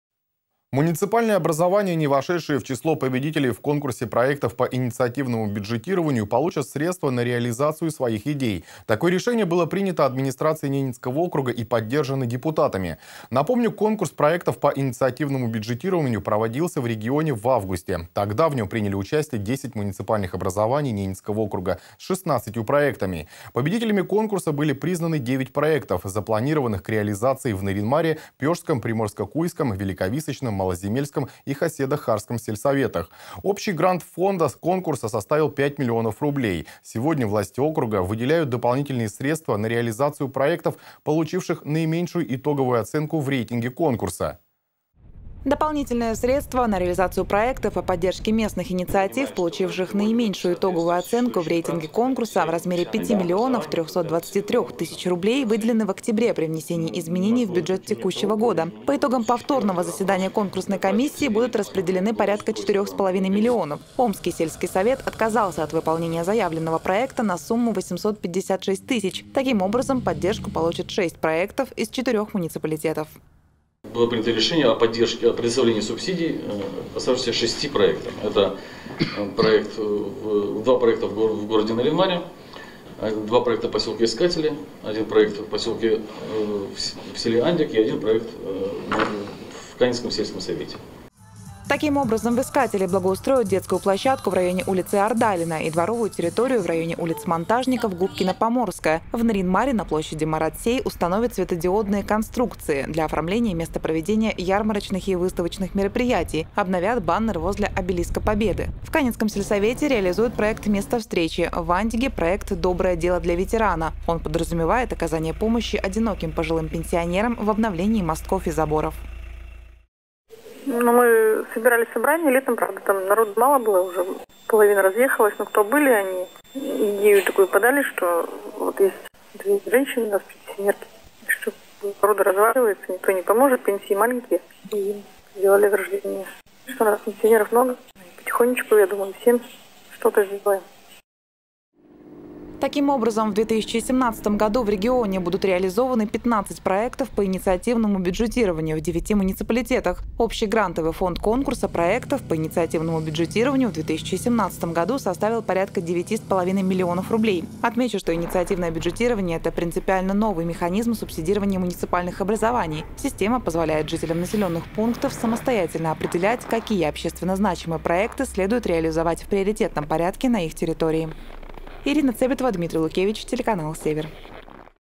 Все окружные проекты конкурса по инициативному бюджетированию получат средства на реализацию идей
Муниципальные образования, не вошедшие в число победителей в конкурсе проектов по инициативному бюджетированию, получат средства на реализацию своих идей. Такое решение было принято Администрацией Ненецкого округа и поддержано депутатами. Напомним, конкурс проектов по инициативному бюджетированию проводился в регионе в августе. Тогда в нем приняли участие 10 муниципальных образований Ненецкого округа с 16 проектами. Победителями были признаны девять проектов, запланированных к реализации в Нарьян-Маре, Пешском, Приморско-Куйском, Великовисочном, Малоземельском и Хоседо-Хардском сельсоветах. Общий грантовый фонд конкурса составил 5 млн рублей. Сегодня власти округа выделяют дополнительные средства на реализацию проектов, получивших наименьшую итоговую оценку в рейтинге конкурса.